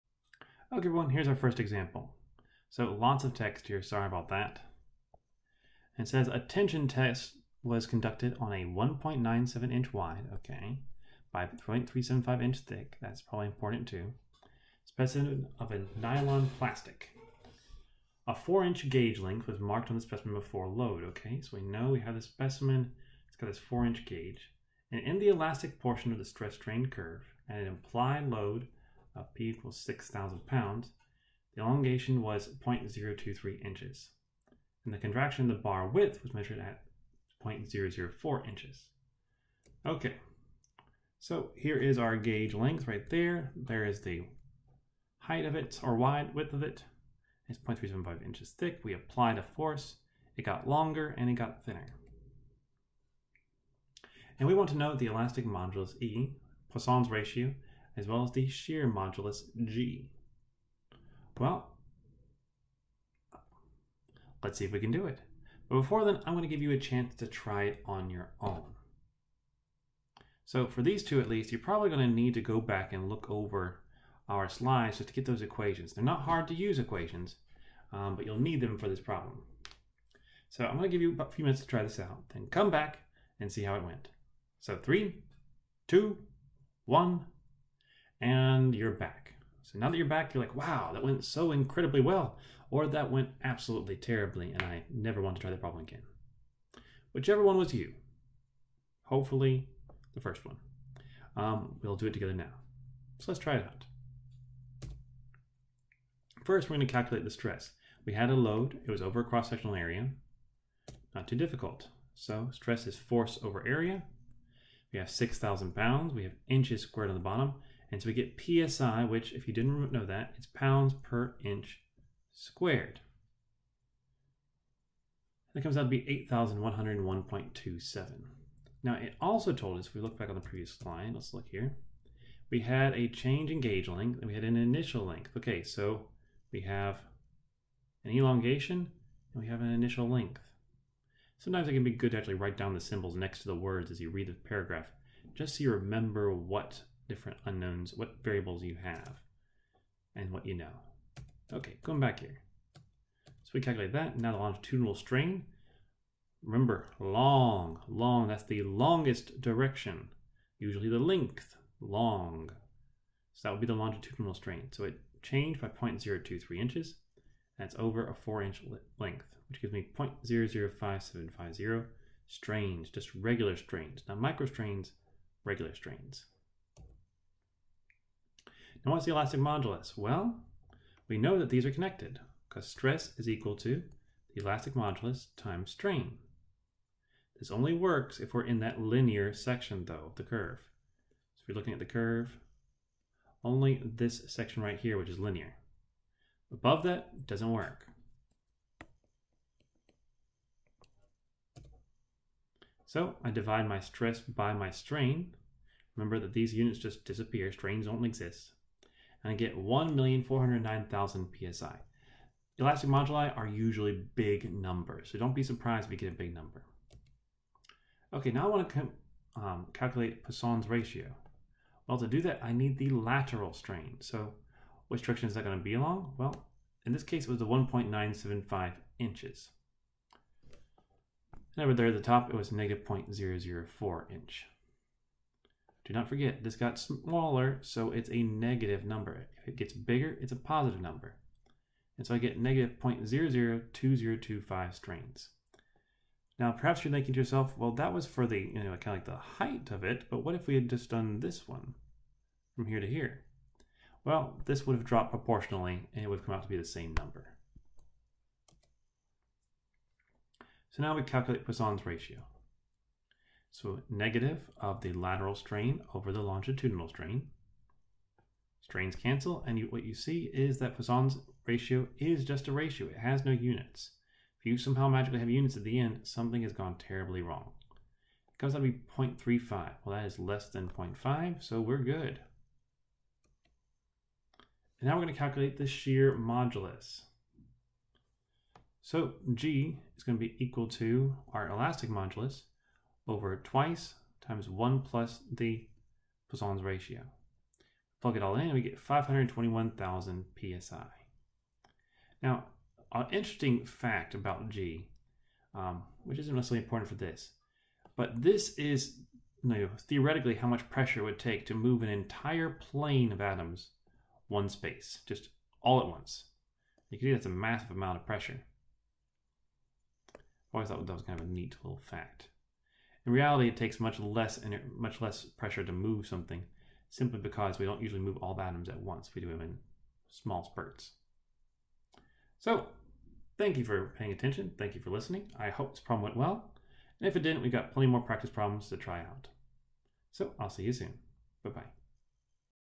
Example: Calculating Poisson's Ratio
Figuring out Poisson's ratio isn't terribly difficult. It is after all a...ratio. That just means division. Take the strain in the short direction and divide it by the strain in the long direction. Voila. Oh and make sure its positive.